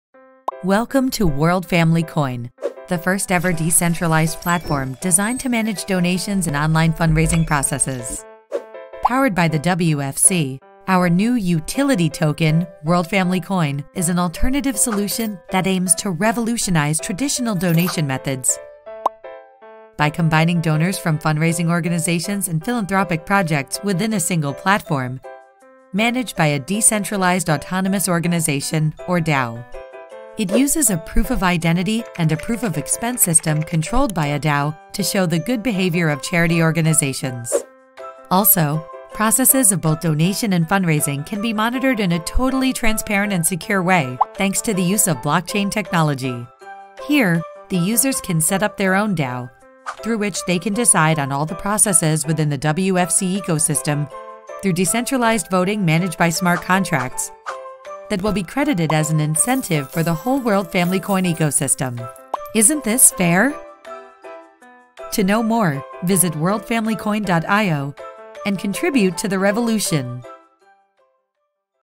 World Family Coin | 2d Explainer Video | Doodlemango
Team DoodleMango is happy to share with you the World Family Coin Explainer video that conveys the cause and educate the viewers about the process and benefits!